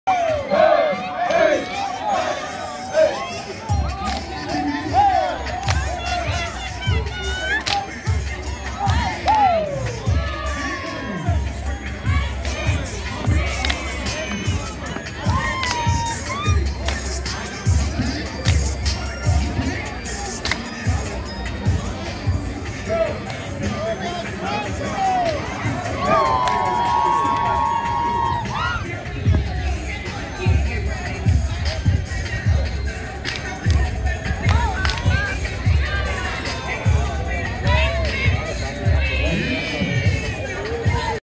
Phi Mu Alpha/Sigma Alpha Iota Stroll
Phi Mu Alpha and Sigma Alpha Iota stroll at Music Greeks Weekend 2022.